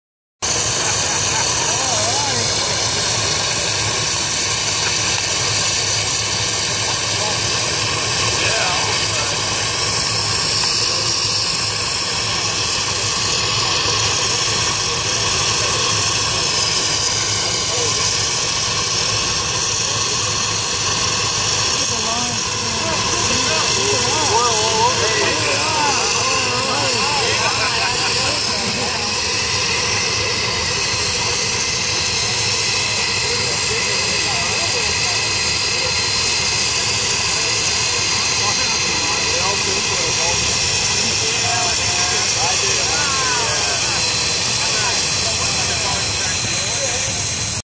John and Jay Forsythe with 1/4 scale Case Traction Engine Spark Show at Gratz 2013
John and Jay Forsythe with 1/4 scale Case Traction Engine at Gratz Antique Machinery Assoc Show July 2013. Frist Spark Show!!!